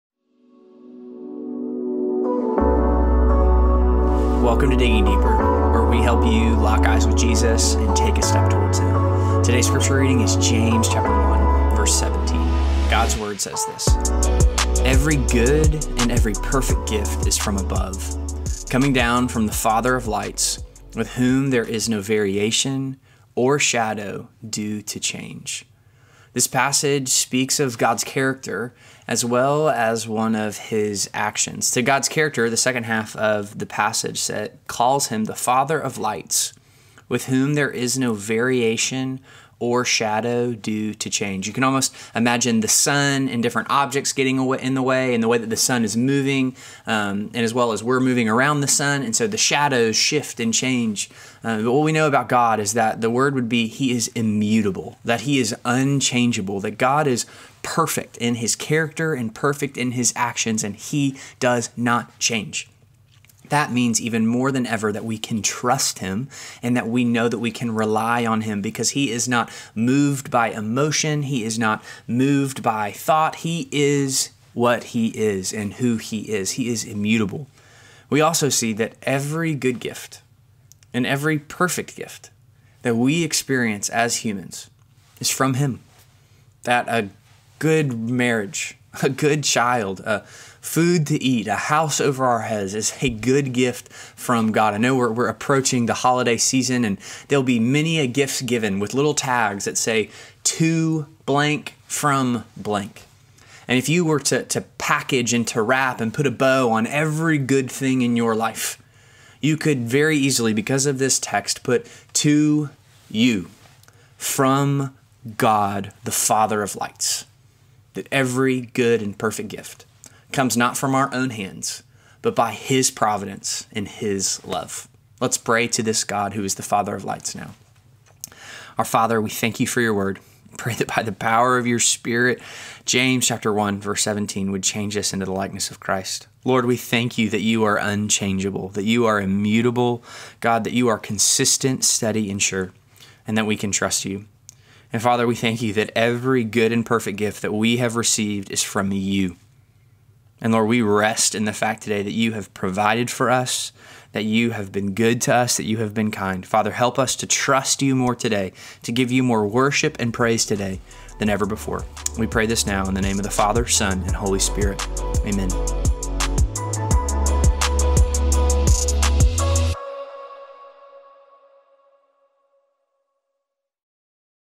Father of Light (James 1:17)
In this episode of Digging Deeper, on week 3 of our How to Get Through What You're Going Through series, we dive deeper into James 1:17.